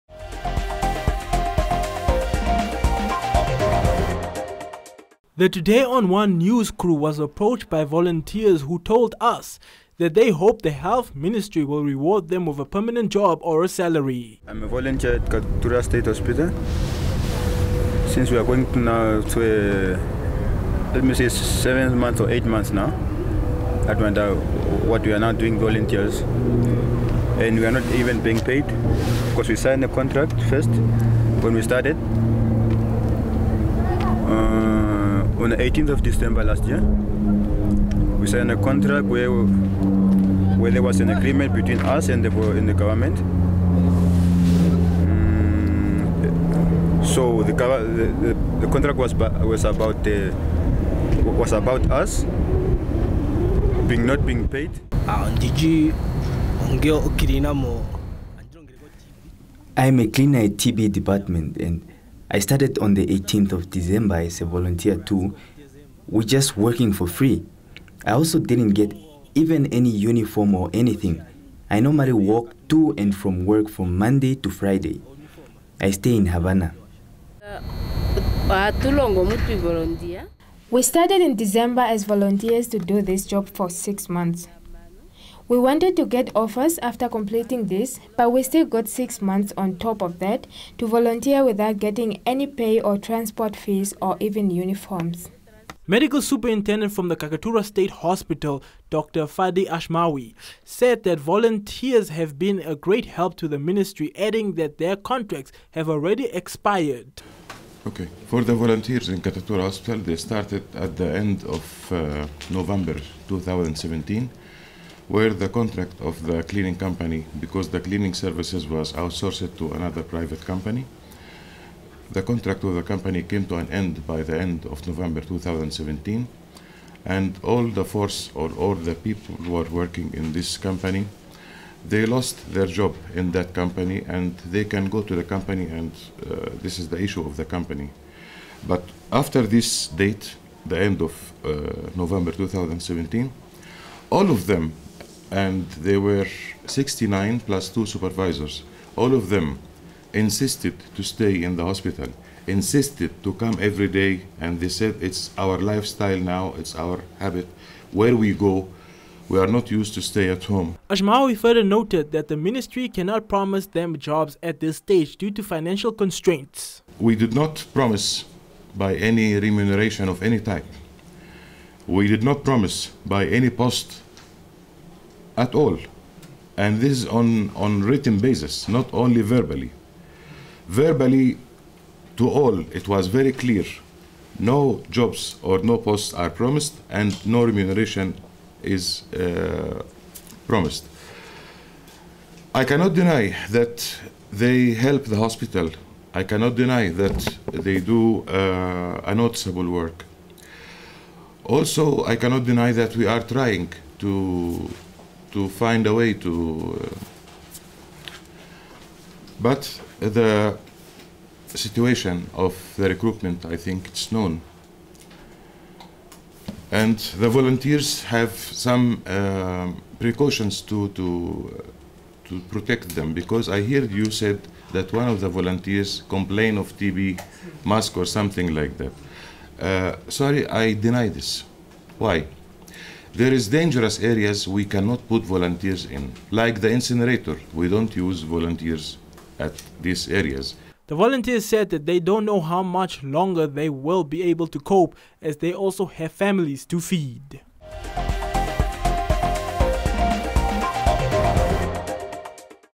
Katutura hospital voluntary cleaners demand payment
by: Leston Wohler ---

Volunteers at the Katutura State Hospital are up in arms as they claim that they have been cleaning the hospital and other health facilities for over a year without being compensated for their services and are now demanding that they be paid.

Medical Superinterdent of the Katutura State Hospital, Doctor Fady Ashmawy said that their contracts have already expired last year and although the Ministry appreciates their work, they can not be permanently employed again, due to financial constraints.